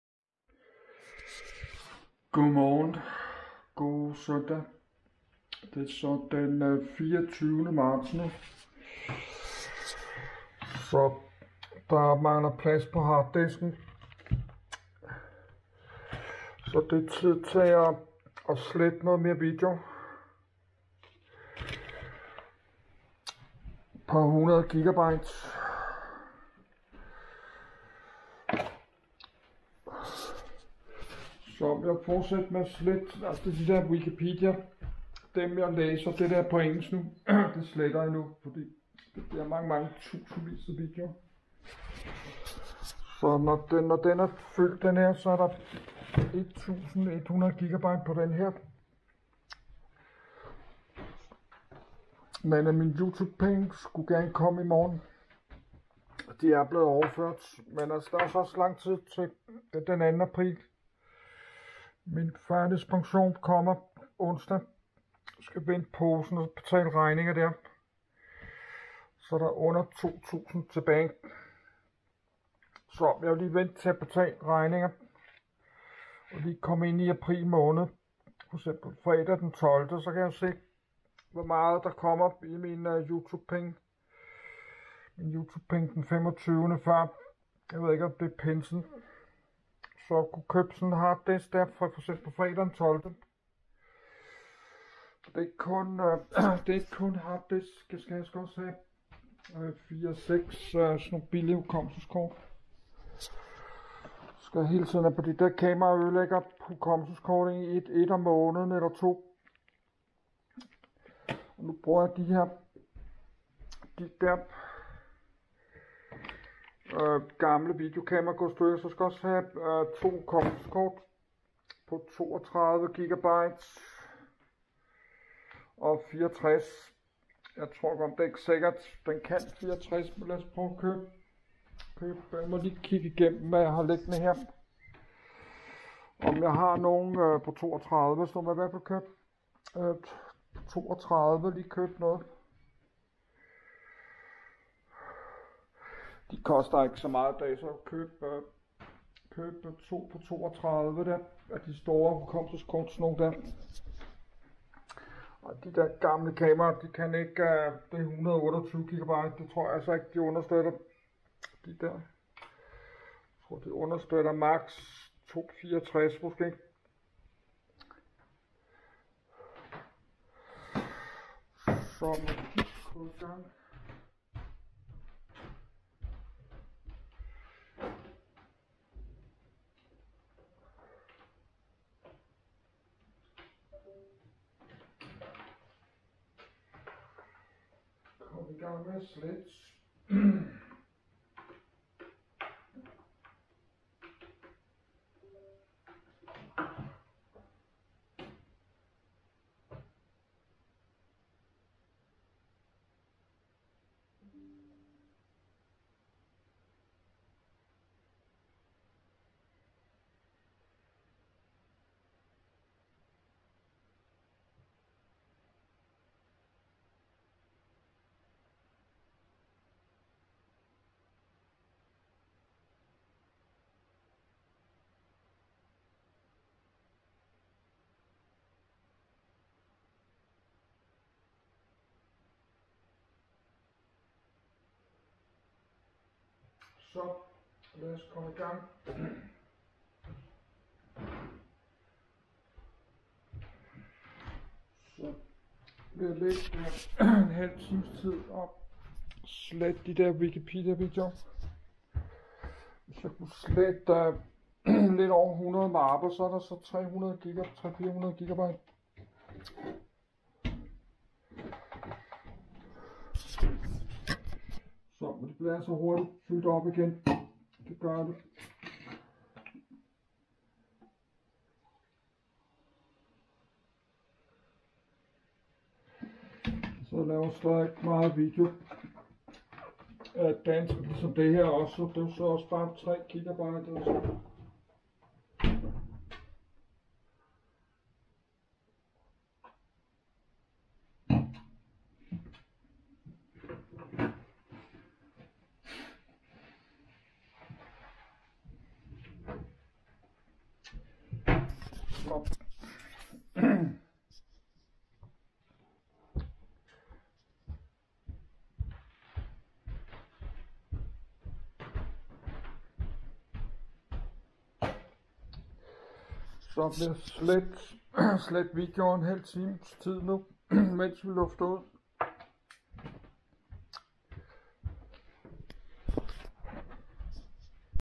MongoTV_13832 - Del 39 - SLETTER MANGE 100 GB ENGELSK WIKIPEDIA VIDEO FOR SPARE HARDDISK PLADS
DEL 39 - God SØNDAG. Der er snart under 50 GB plads tilbage, på min ekstern USB harddisk, hvor jeg gemmer en original kopi, af mit dansk og engelsk video. Men jeg fik ligget en 40-45 minutter nu, og slettet de engelsk wikipedia video. Imens jeg luftet ud, fra lørdag nat. Nu er der lukket igen, og varme sat på igen. Der er cirka 690 GB plads nu, og er ca. 310 GB mere jeg kan slette, og cirka 340 GB af noget engelsk Airlinesim webbrowser spil video, som jeg nok også, bare kan slette. Så jeg må lige igen, kigge de andre eksterne USB harddisk, igennem igen, om der skulle være nogle 100 GB mere, af det engelsk Airlinesim spil video, som jeg kan slette også. Jeg har under 2.000 kr. md. tilbage, i April måned, af penge fra min førtidspension. Så jeg har stadig, for lidt penge, at leve af nu. Omkring fredag den 12.April 2024, der kan jeg nok se igen, saldo på mine næste YouTube penge, der kommer omkring den 25.April. Men jeg tror bare, at jeg skal så købt, en ny dyr 5.000 GB ekstern USB harddisk, til omkring 1.000 kroner, og for en 300 kroner, nogle blandet billig små 32-64 GB hukommelskort, til mine gamel videokamera. Men nu er klokken 10.30, og jeg vil gerner lave noget andet nu.